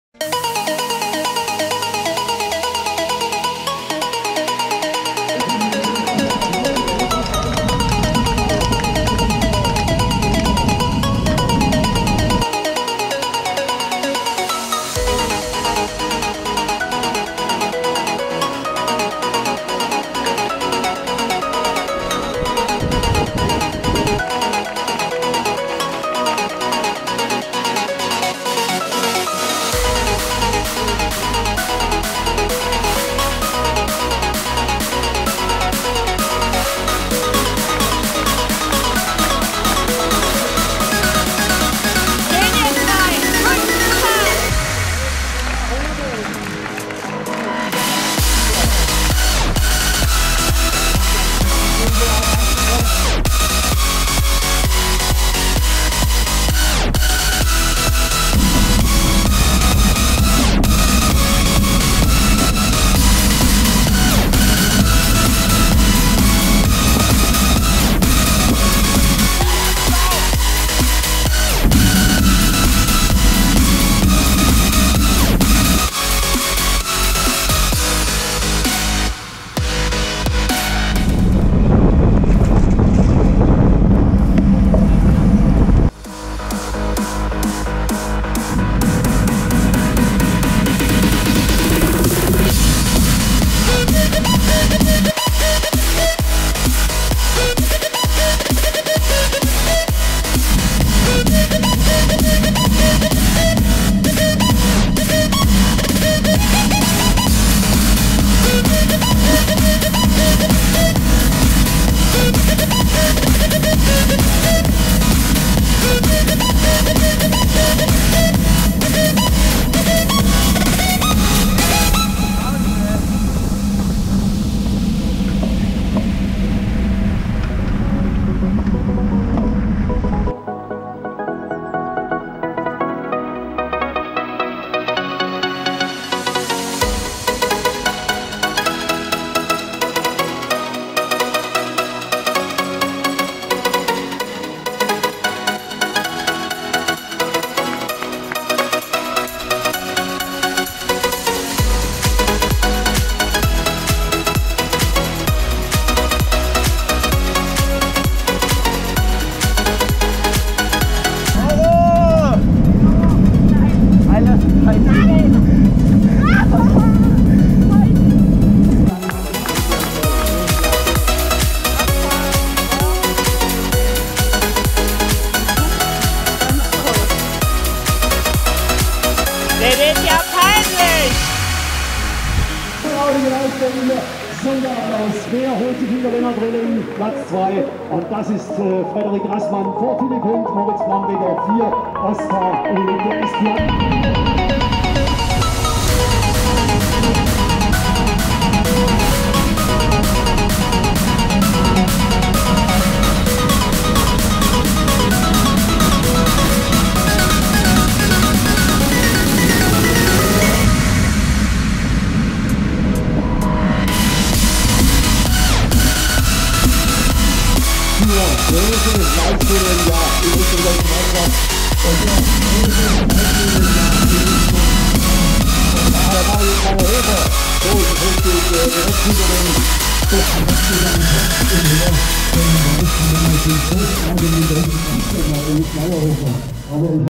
Rund um Schönaich Junioren U19 02.04.2018
Das Rennen gewann Lennart Rilling.

© Henri und Oscar Uhlig (Cyclocross Portal)